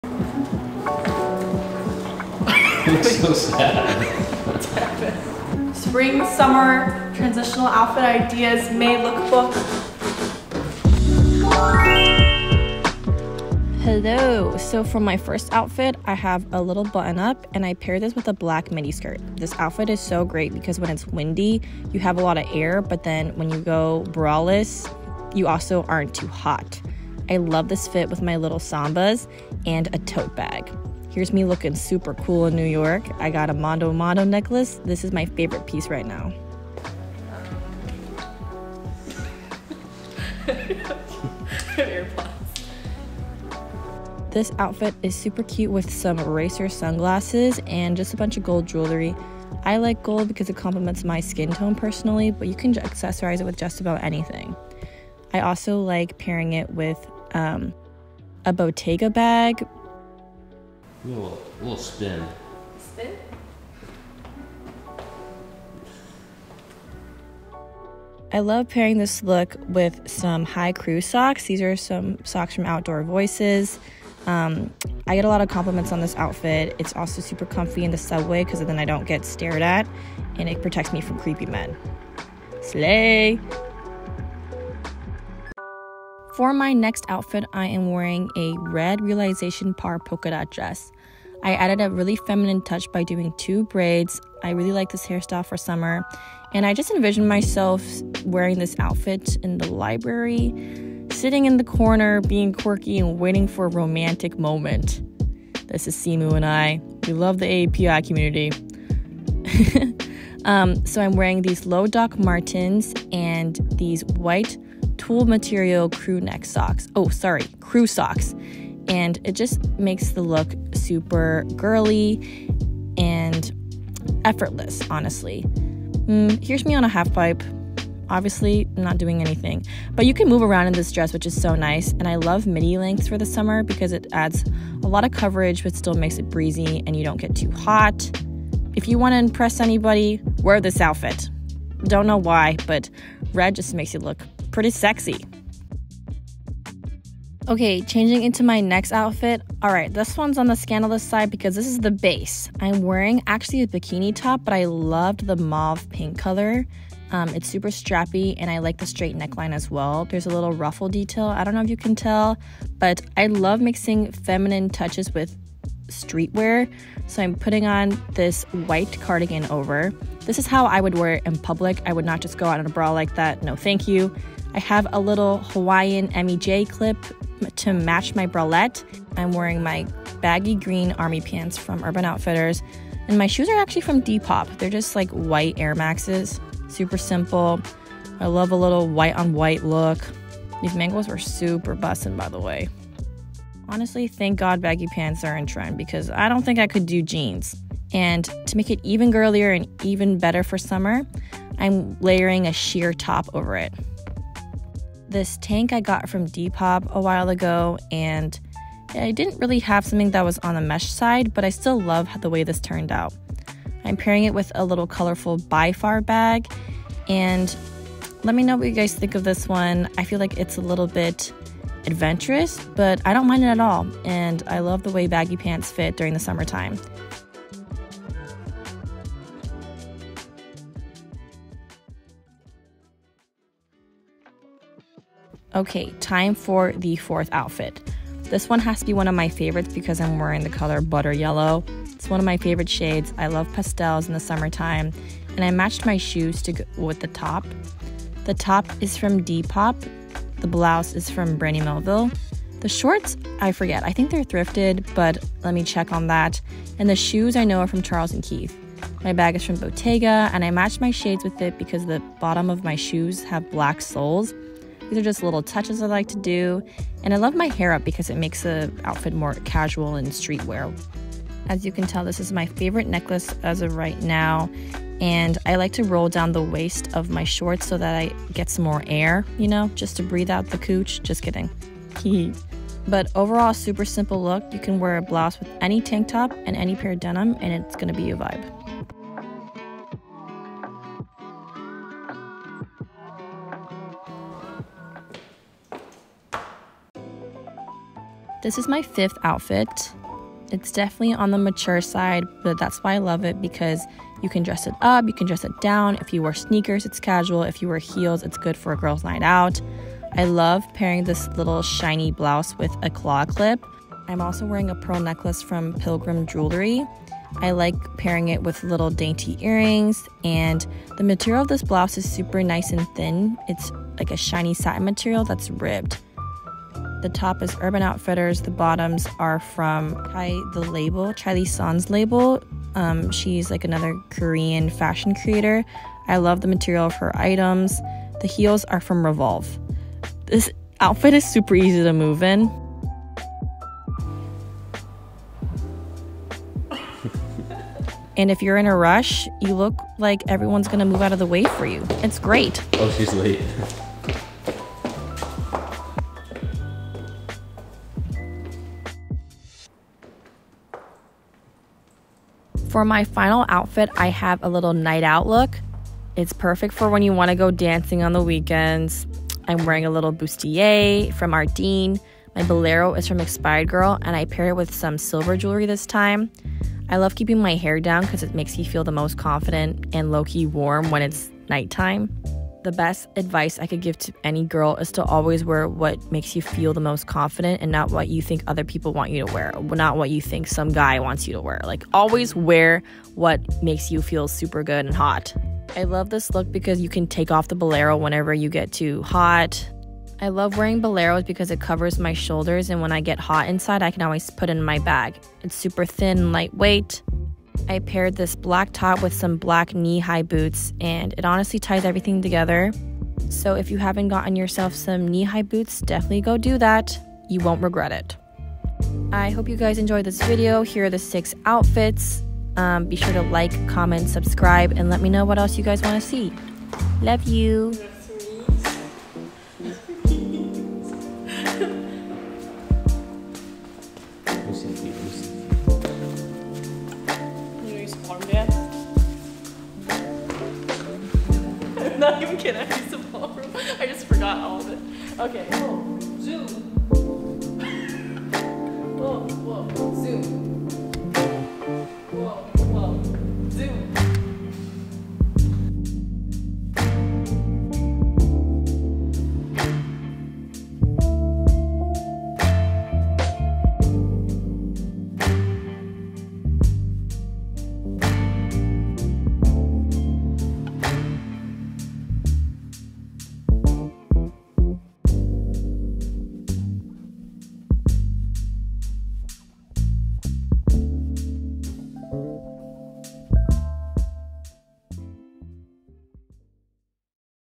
fun summer outfit ideas ✿ | spring to summer transitional looks
sorry sisters i could not do a voiceover without a little stutter
hope u enjoyed the outfits :)
let me know what else you want to seeeeee

thank you @LukeEich1 for helping film ✨

title card animation by nicole katherine